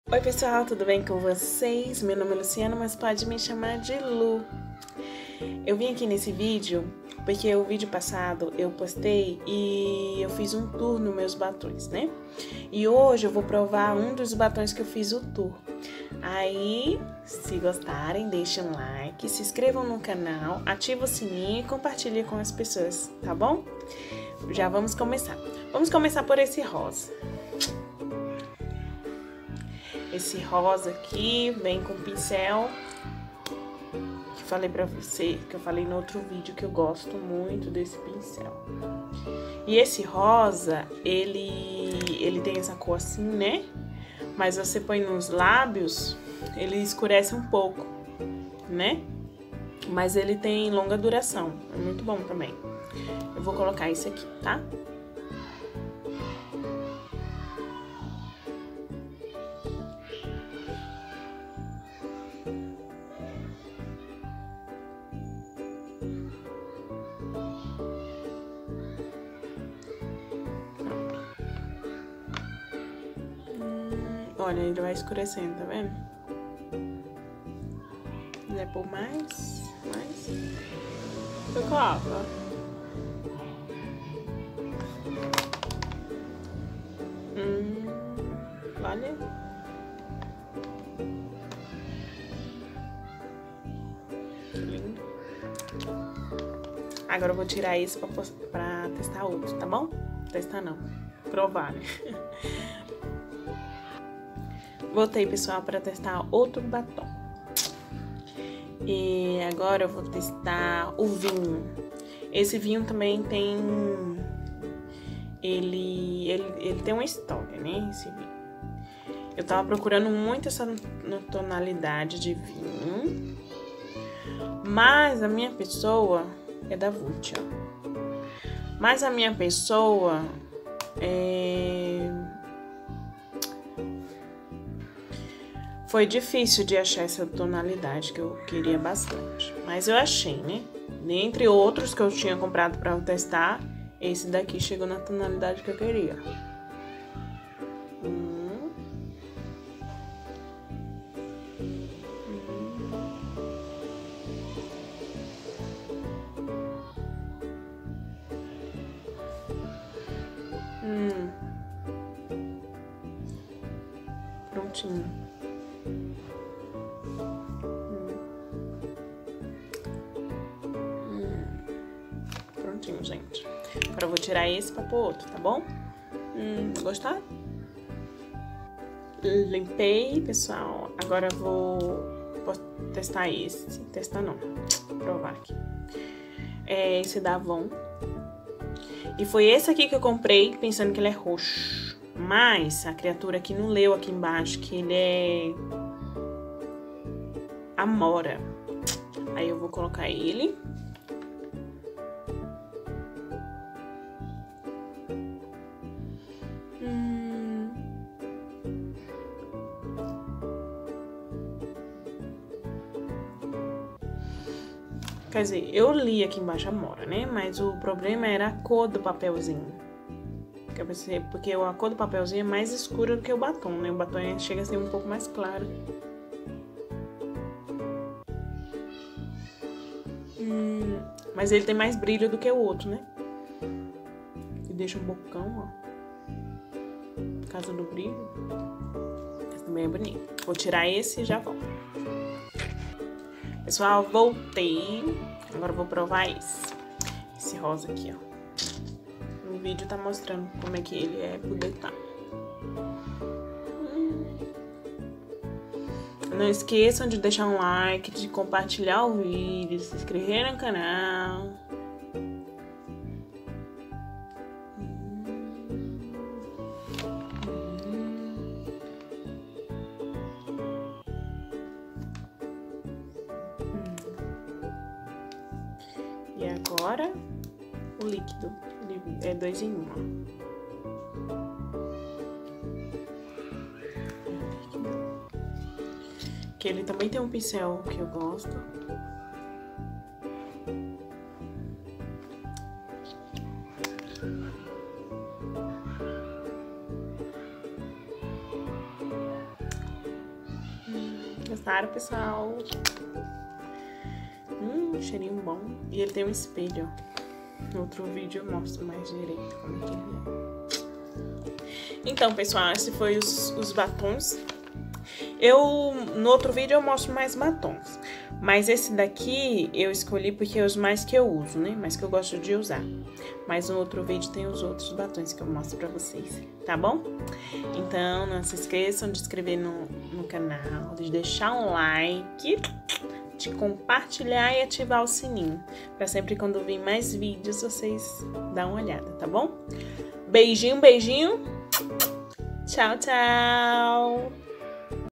PROVANDO BATOM VULT AVON
olá pessoal tudo bem com vocês? nesse vídeo eu estou mostrando meus batons favoritos  ,espero que gostem da cor e da indicação. São batom da avon e vult e de outras marcas ,tem no tom rosa e amora na cor 15 também ,desde ja obrigada por estarem aqui😉

DICAS QUE VIRÃO :

-dica de sobrancelhas...
- dica de olho ...
-dica de batom ...
-dica de roupa ...
-dica de cabelo
-teste de produtos
-dicas de delineador

dia a dia